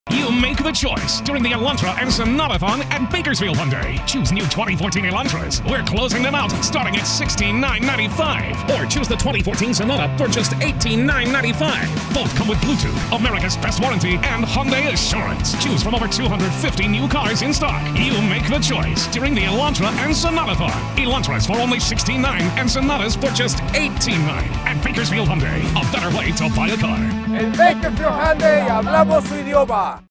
Brand new 2014 Sonata or Elantra at Bakersfield Hyundai
Join Bakersfield Hyundai during our Elantra and Sonatathon! Elantras starting at $16,995, Sonatas starting at $18,995. Both come with Bluetooth, America's best warranty, and Hyundai Assurance.